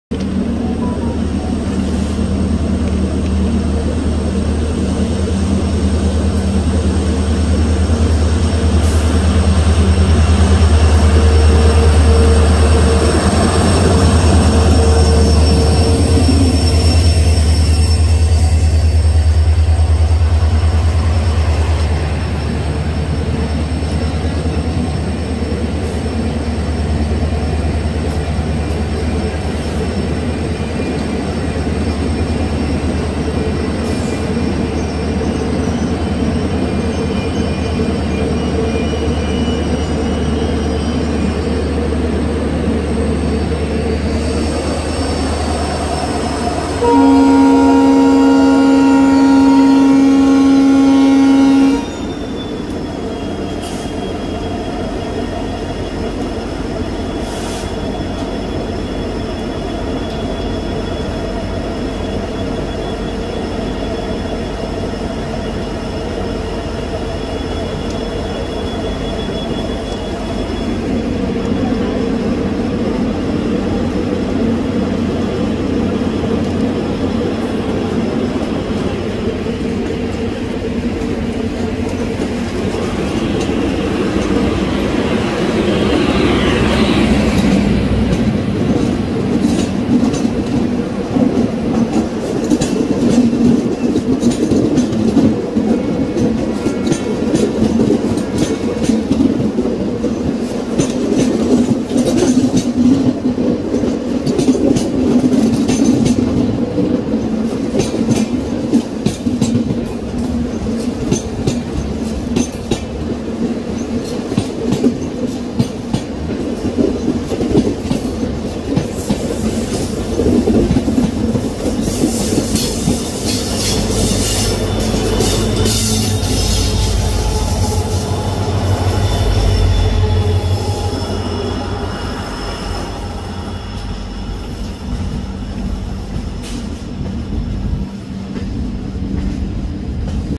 KARWAR VISTADOME CROSSING BANGALORE VISTADOME
Karwar express from bangalore is crossing Karwar express coming from Mangalore